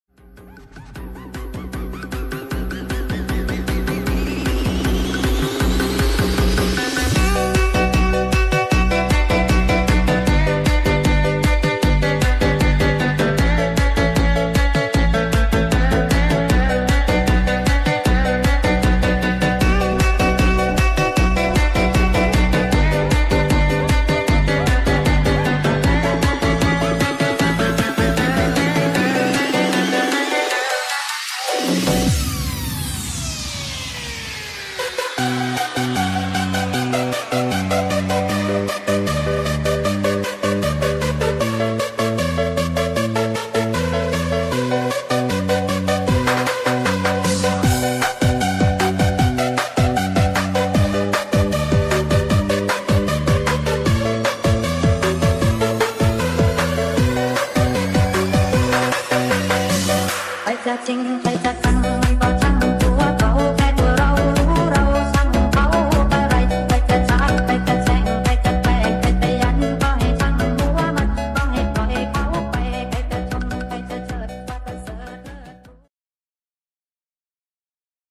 SAWADIKAP KAP KUN KAP!!!TaiThai Seafood Larkin Perdana Ajommm
berbagai menu Dari Thailand mmg Padu 💛💚❤️🔥🔥🔥